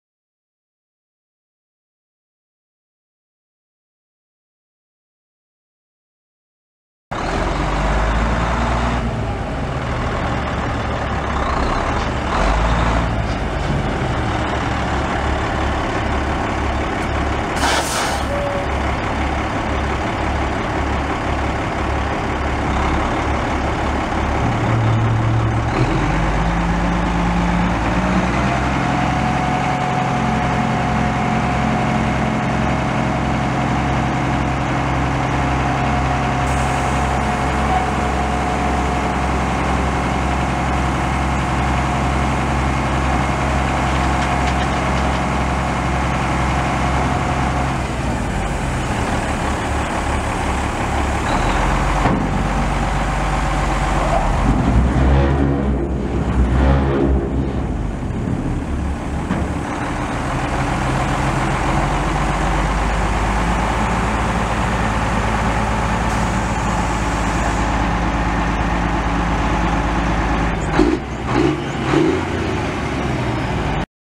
1997 Freightliner FLD112 dump truck for sale | sold at auction December 18, 2014
SOLD! NoMug Construction Estate Auction

1997 Freightliner FLD112 dump truck
355,748 miles on odometer
Caterpillar 10.3L L6 diesel engine
10 speed manual transmission
Twin screw
AC and heat
Tilt
Cruise
AM/FM/cassette
Davis dump bed
12 to 15 yard capacity
Air end gate
Bed vibrator
Three stage single ram hoist
Exterior visor
Chrome bumper
Block heater
140 gallon fuel tank
PTO
Air suspension
Air brakes
Jake brake
Brake controller
206" wheelbase
295/75R22.5 tires
Unit# 2
1FUY3EDB0VP837907